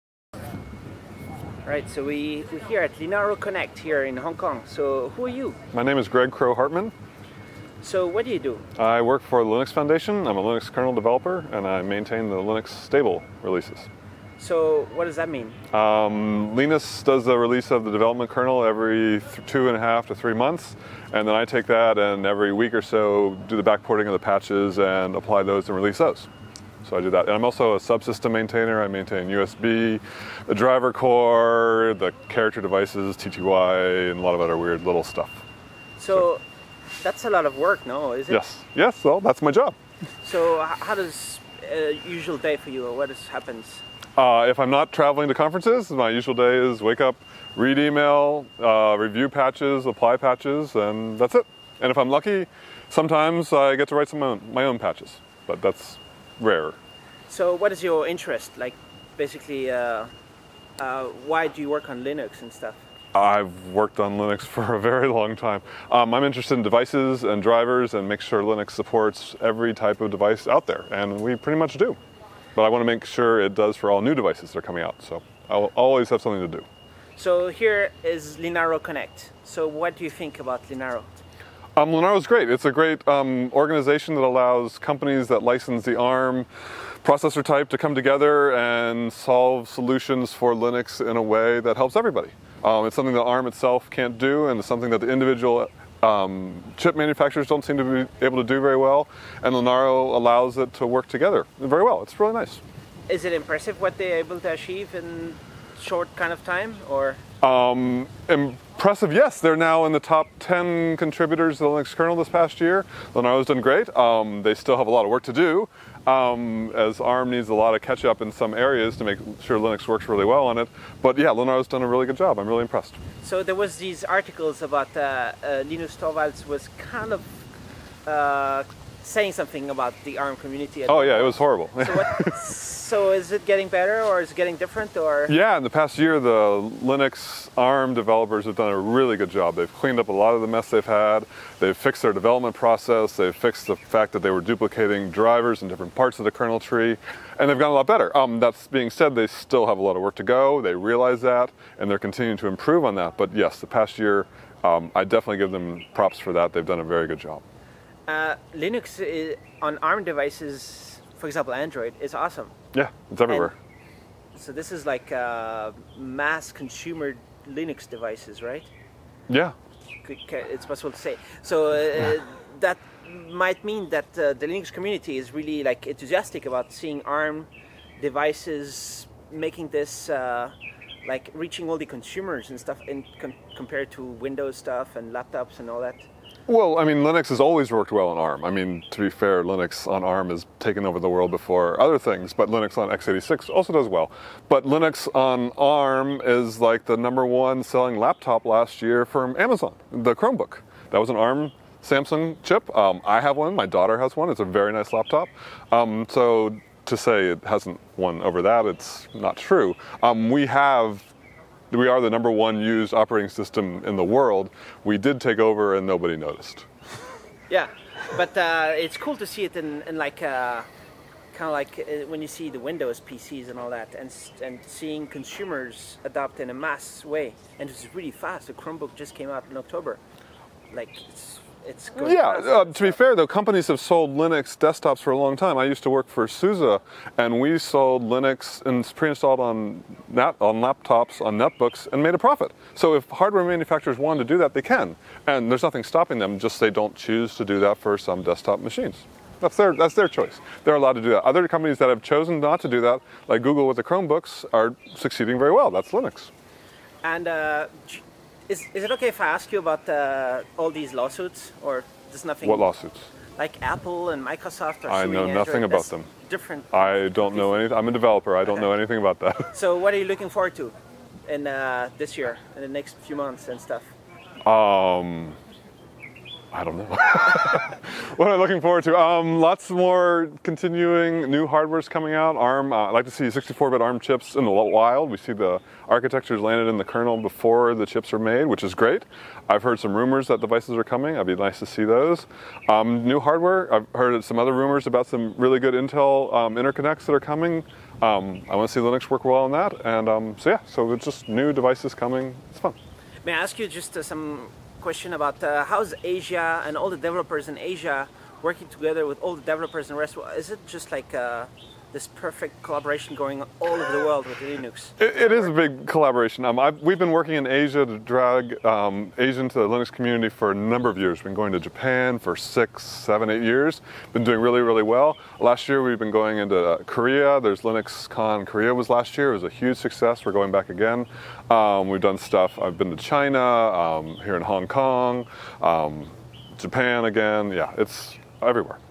Greg Kroah-Hartman at Linaro Connect 2013
Greg Kroah-Hartman of the Linux Foundation, Maintainer of the Linux Stable releases, talks about what he is doing at Linaro Connect 2013.

Filmed at Linaro Connect 2013 in Hong Kong using the Panasonic GH3 camcorder with 12-35mm lense on a Tiffen Steadicam Merlin 2 with vest